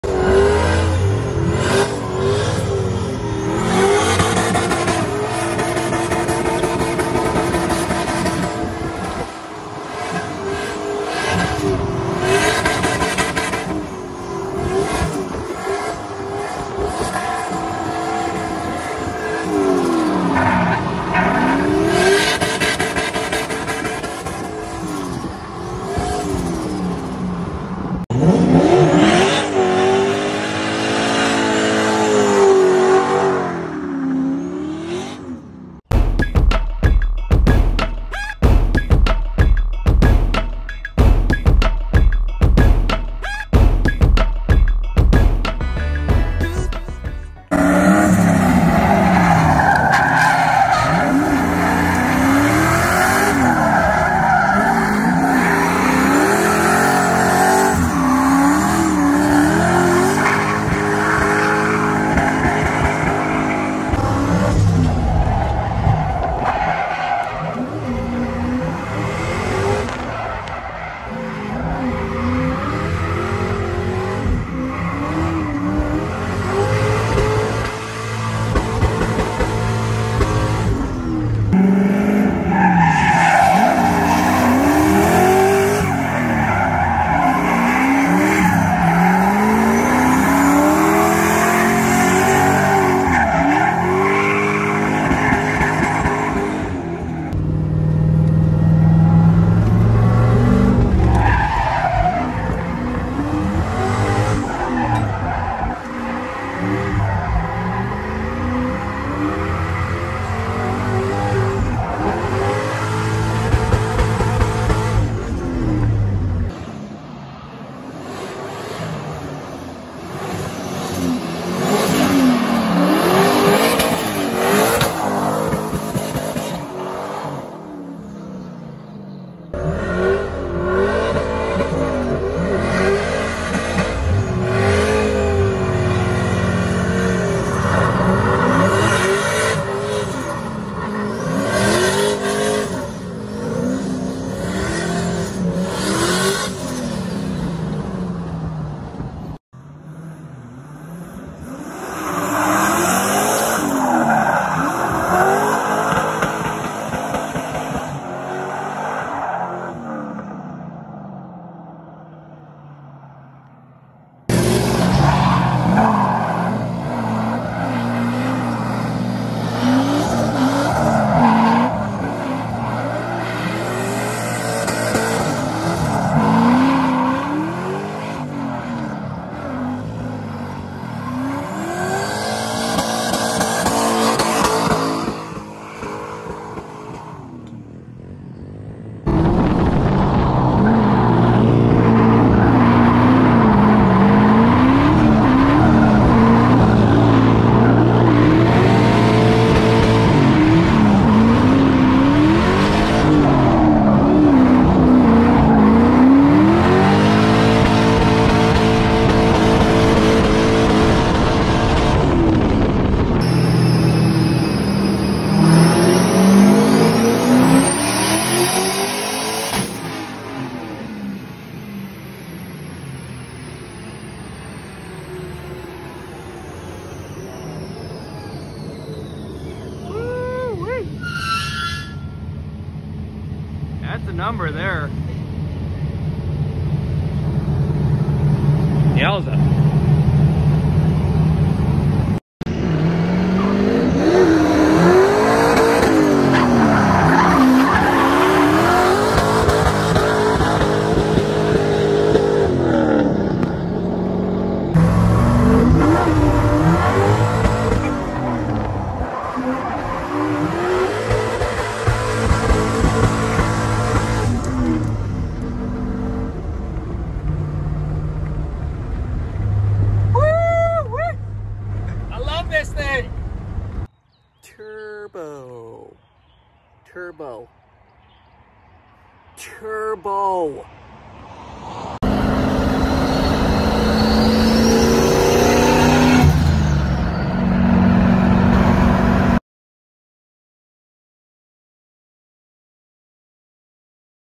The ultimate Silverado drift truck. Turbo Chevy truck street drifting compilation
You would be hard pressed to find another Silverado drift build like this one.. the truck is set up like a lot of drift cars with a hydro e brake and modified steering angle, but can also tow aswell. Give the people what they want.. 5 minutes of tire smoke and rev limiter. Enjoy!!